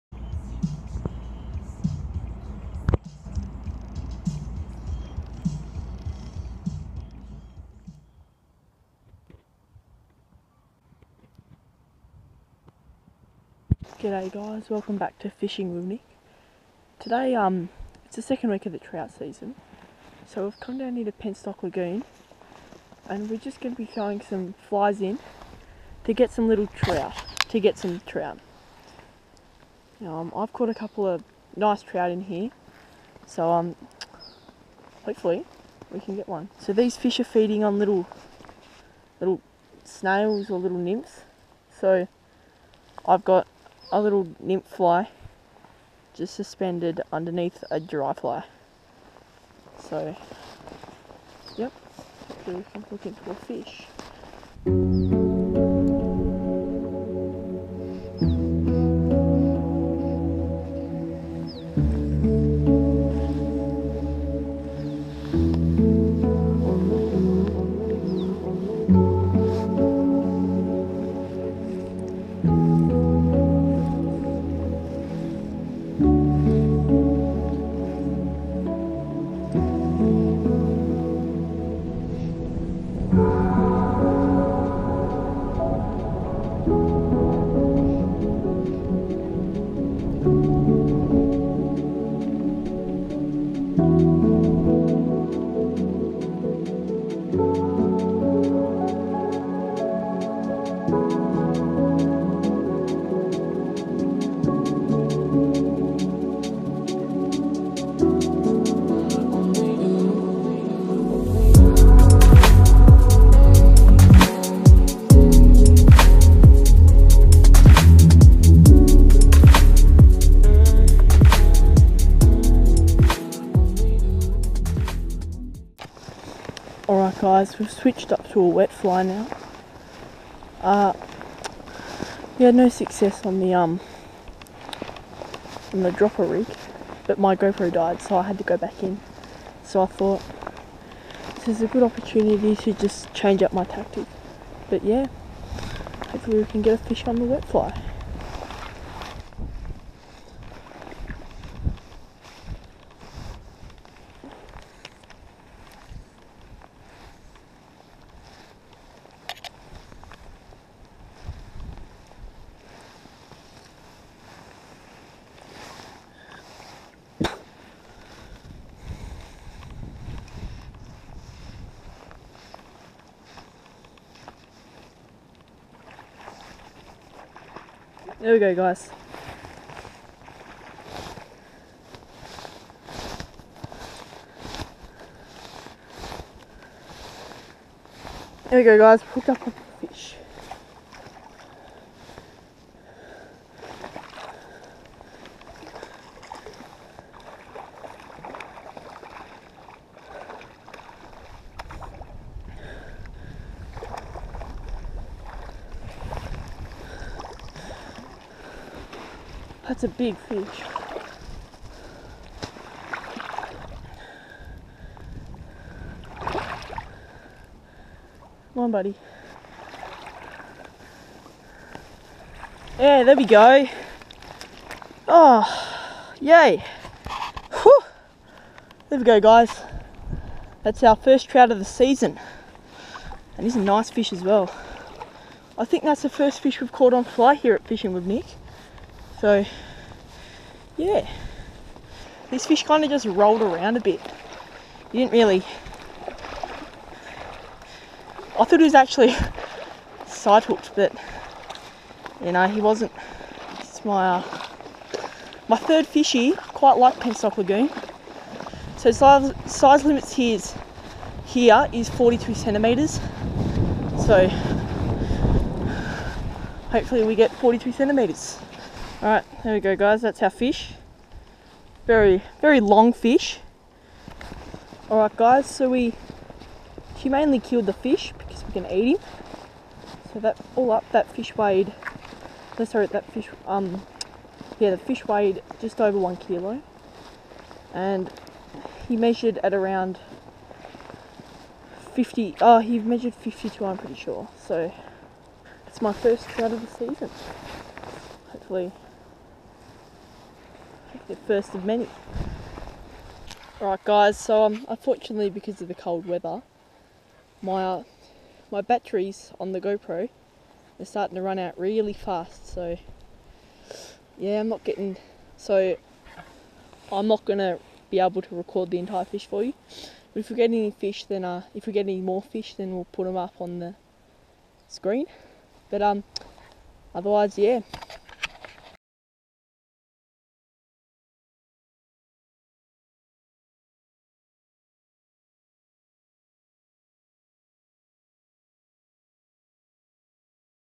Penstock Lagoon Trout Fishing
Yay, I finally caught my first trout of the season. In this vid dad and I venture up to Penstock Lagoon to try our luck at catching some Trout.  After fishing for around 3 hours and catching one fish we decided to head home. This was partly because our fingers felt like they were going to fall off